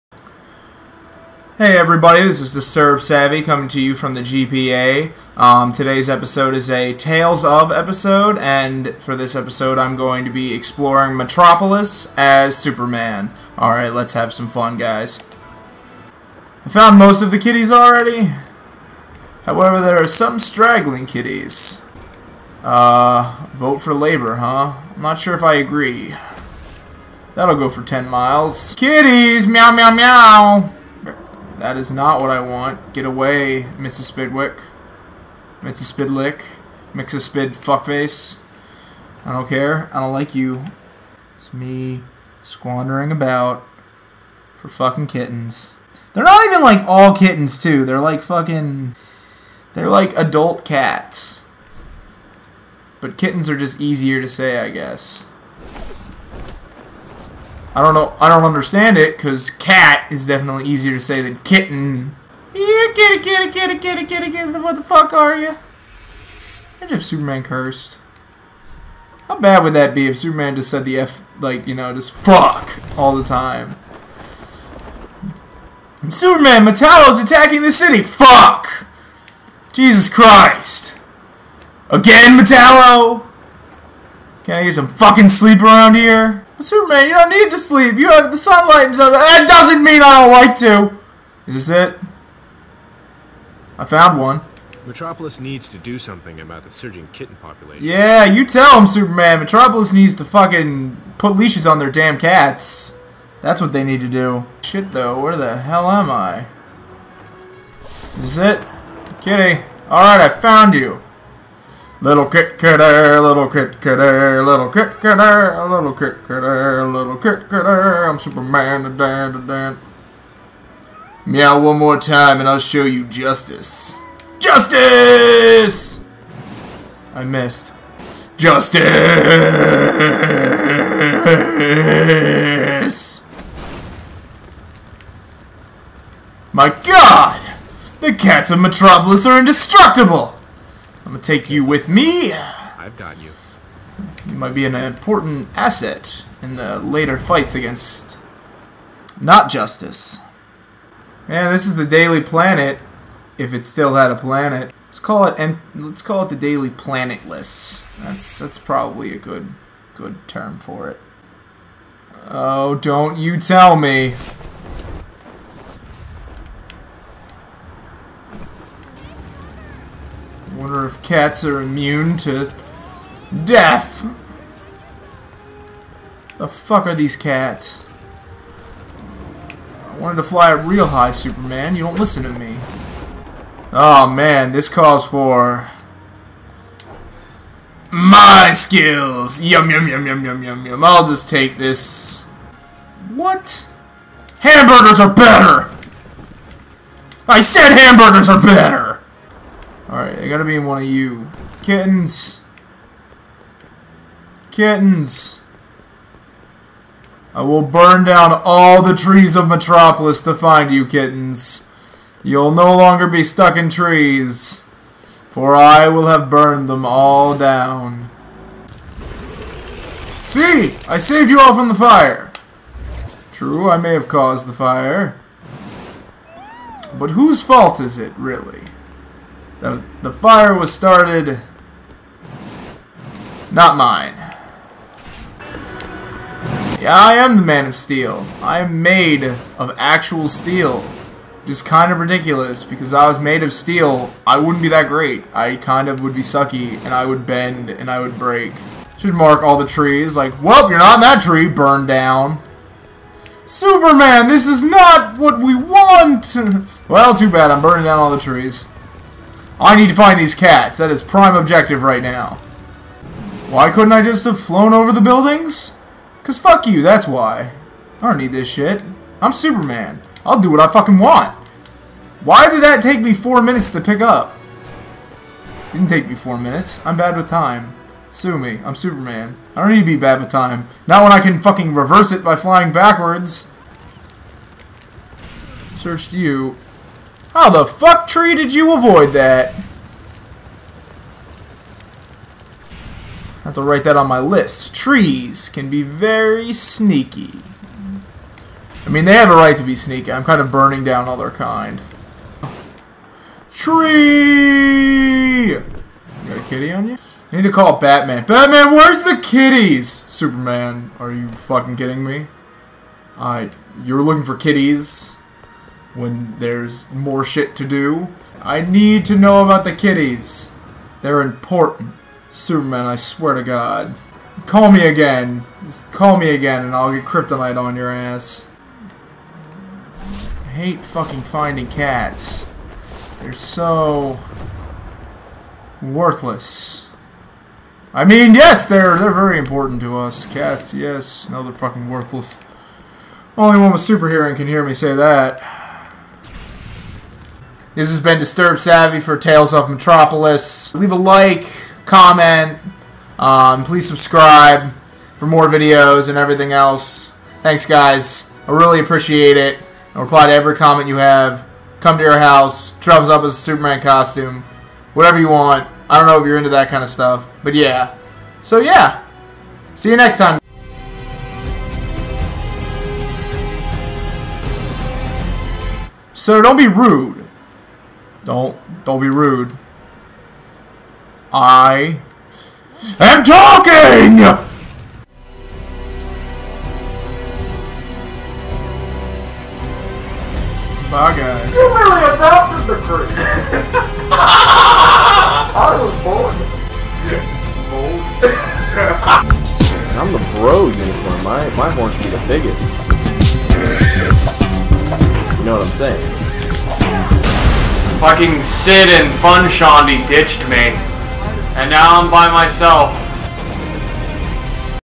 Tales Of: METROPOLIS Part 2 (s)
We join DisturbedSavyy yet again as Superman soaring through Metropolis! This episode he focus' on finding them Kitties/Cats. Even if he has to burn down all the trees to do it!